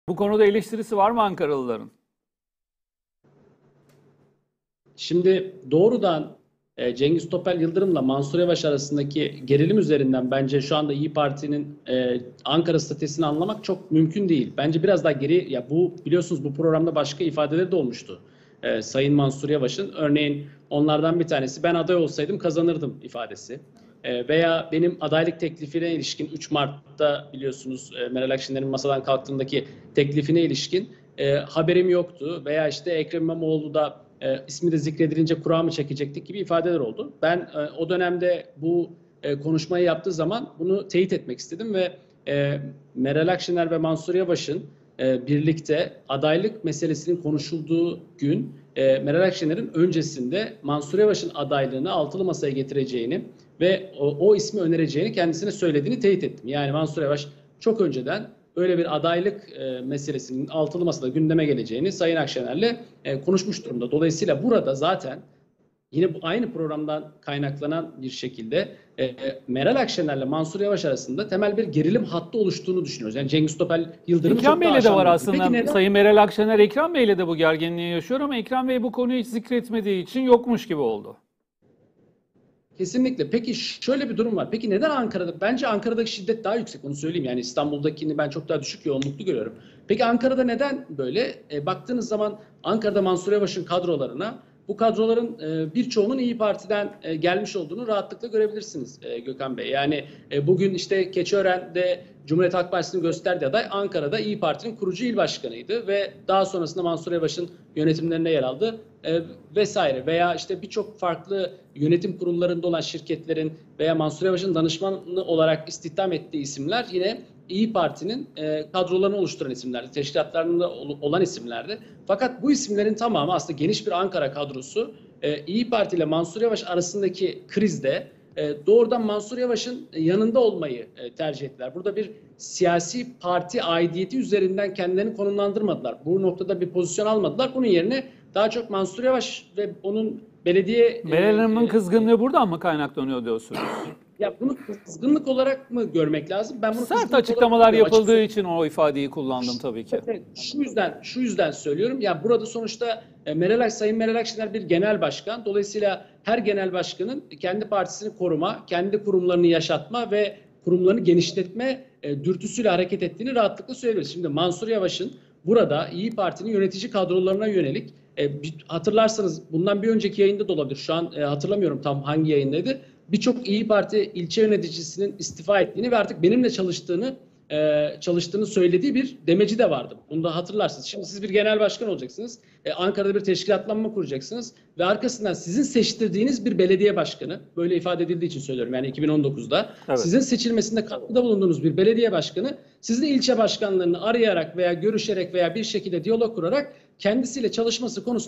İYİ Parti'nin Ankara'da yürüttüğü strateji ne? İlteriş Ergun açıkladı!
Digiturk 62.

D-Smart 147.